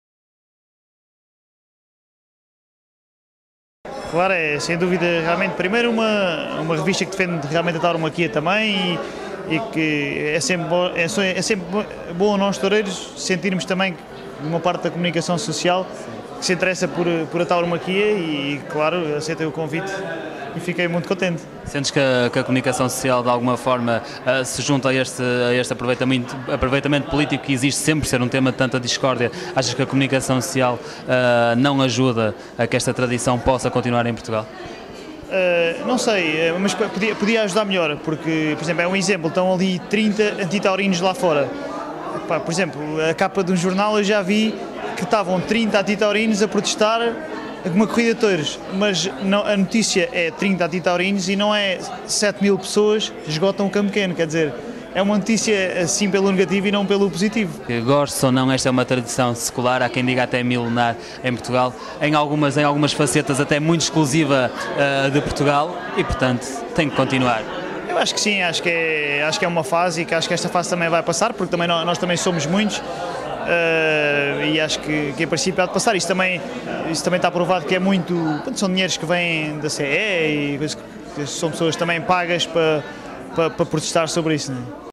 FLASH!TV: Com António Maria Brito Paes na Corrida FLASH! no Campo Pequeno
A FLASH!TV entrevistou António Maria Brito Paes na Corrida FLASH! que decorreu no Campo Pequeno.